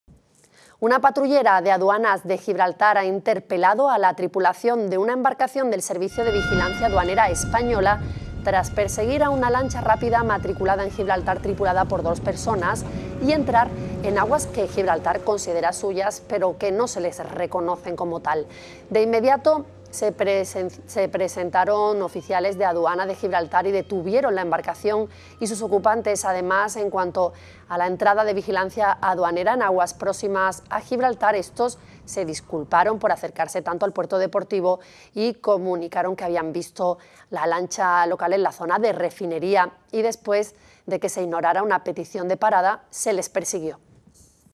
Gibraltar pide explicaciones a Vigilancia Aduanera por perseguir una lancha "en aguas británicas"
Una patrullera de Aduanas de Gibraltar ha interpelado a la tripulación de una lancha del Servicio de Vigilancia Aduanera española tras perseguir a una lancha rápida tripulada por dos personas y entrar "en aguas territoriales británicas de Gibraltar". Según ha informado un portavoz del Gobierno gibraltareño, los hechos se produjeron ayer miércoles sobre las dos y media de la tarde, cuando una lancha del Servicio de Vigilancia Aduanera (SVA) persiguió a una lancha rápida matriculada en Gibraltar.
De inmediato, se presenciaron oficiales de Aduanas de Gibraltar y detuvieron la embarcación y sus ocupantes. Además, en cuanto a la entrada de Vigilancia Aduanera en aguas próximas a Gibraltar, estos se disculparon por acercarse tanto al puerto deportivo y comunicó que había visto la lancha local en la zona de la refinería y, después de que se ignorara una petición de parada, se les persiguió hasta aguas gibraltareñas".